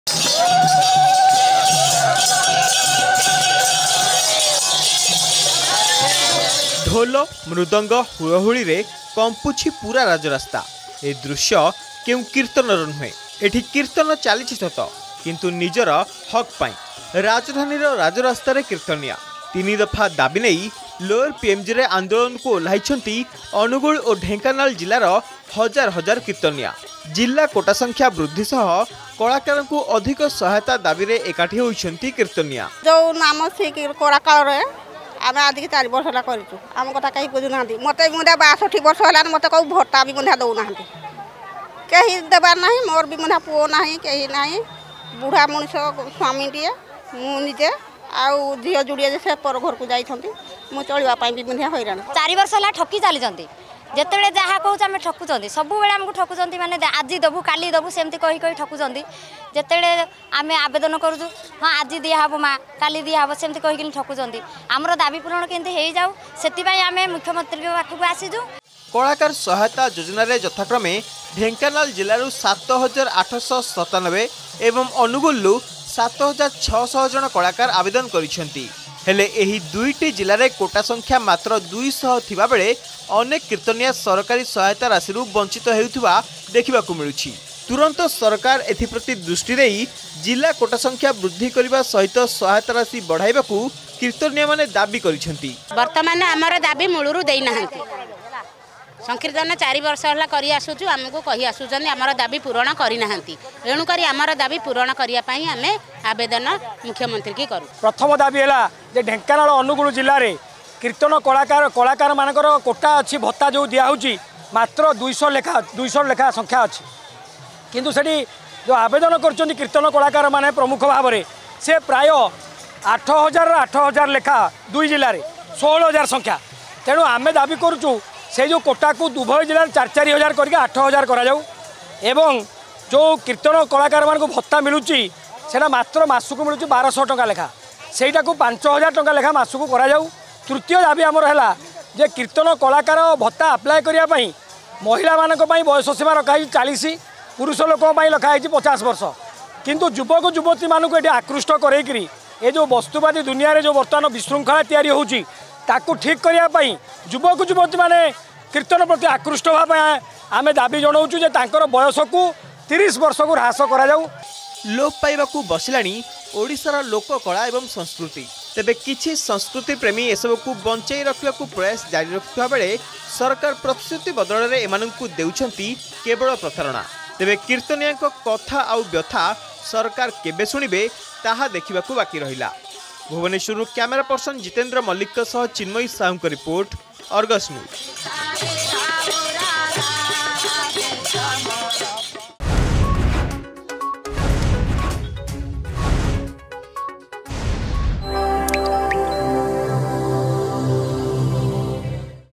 Bhubaneswar: Kirtan Protest by Artists of Angul and Dhenkanal Over Kalakar Sahayata Yojana
Kirtan Protest by Artists of Angul and Dhenkanal Over Kalakar Sahayata Yojana. Artists are Deprived of Govt's Assistance.